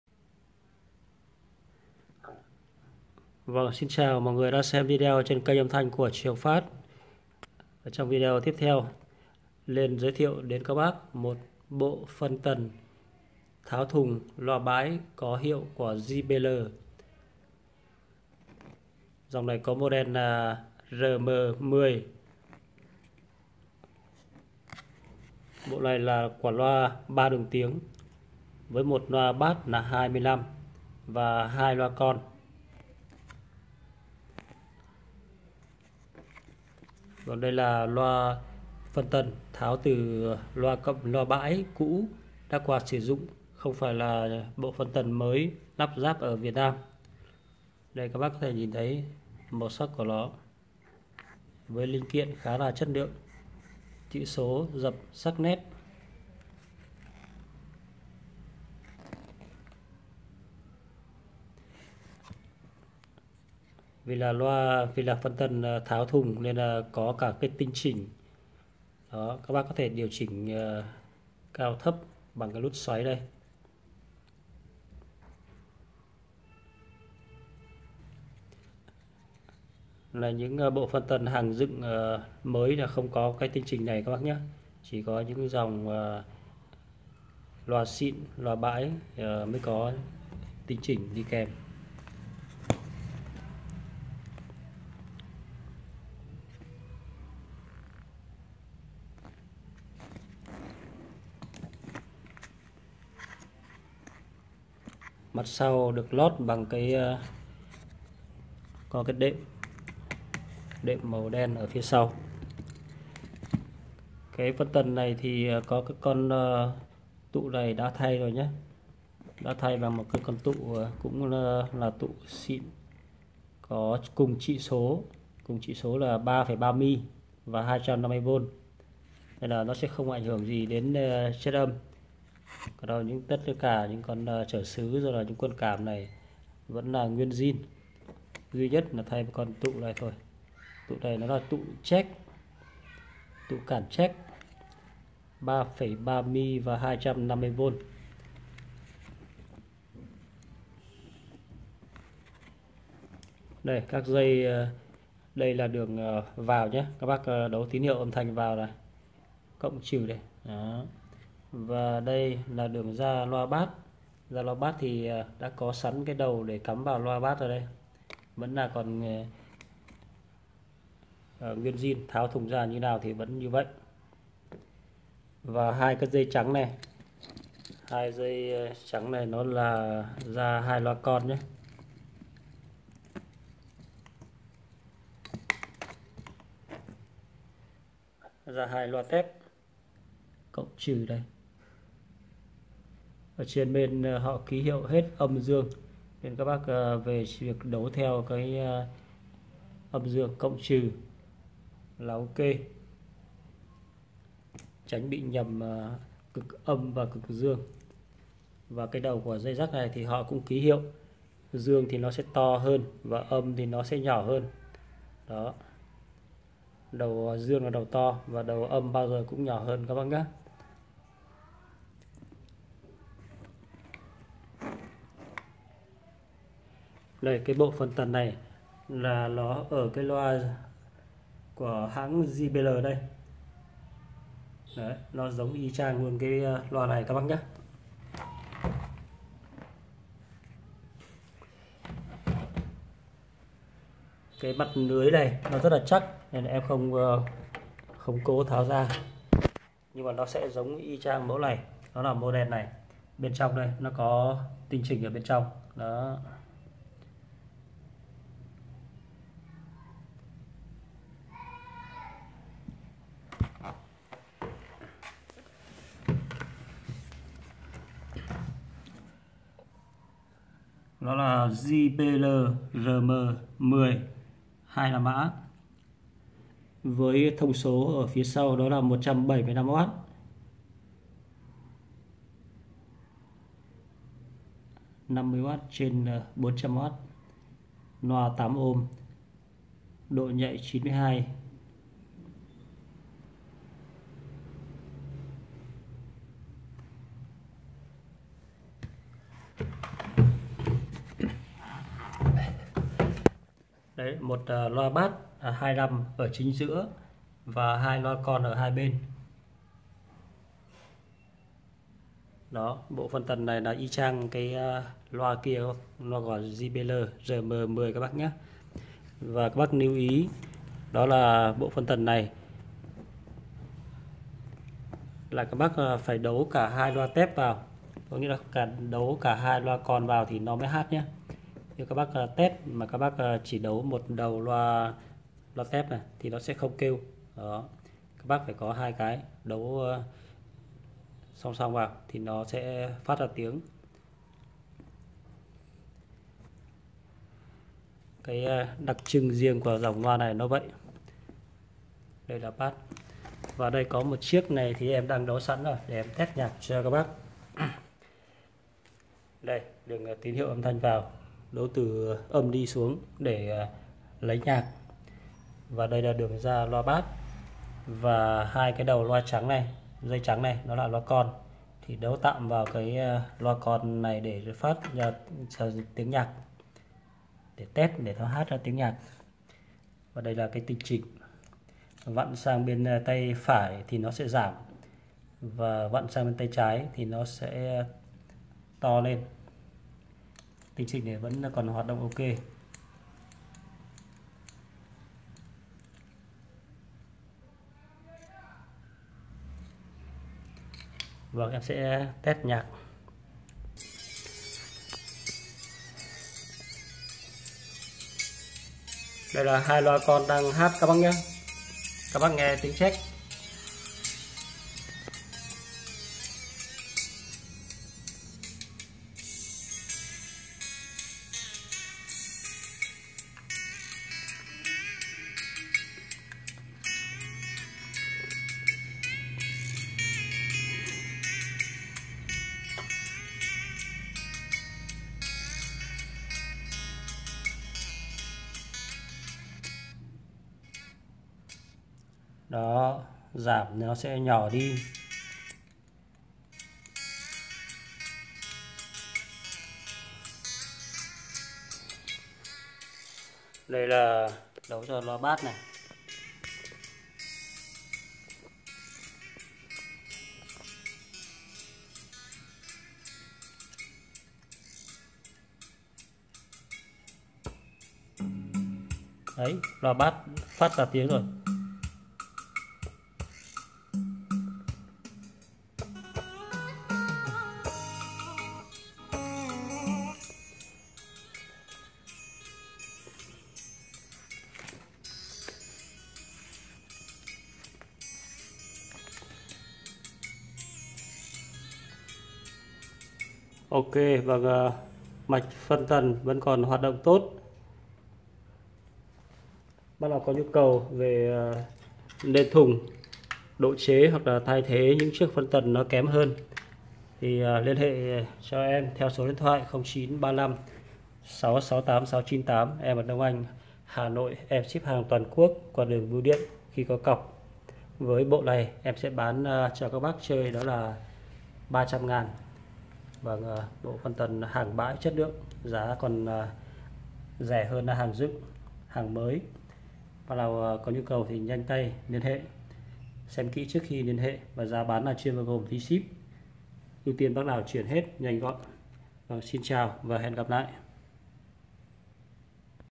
7/9..Bộ phân tần Tháo loa JBL 3 đường tiếng / Lh 0935 668 698 ( zalo)
_ Bộ phân tần tháo  thùng loa JBL RM10 hàng bãi  còn hát tốt, đang dùng cho loa bass 25cm, và 2 loa tep, hàng bãi link kiện chất lượng cao cho chất âm chi tiết , sạch sẽ.. xem video để biết chi tiết..
_ Lh Mr Trường/ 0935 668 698 ( zalo)
_ ĐC/ Đông anh/ Hà nội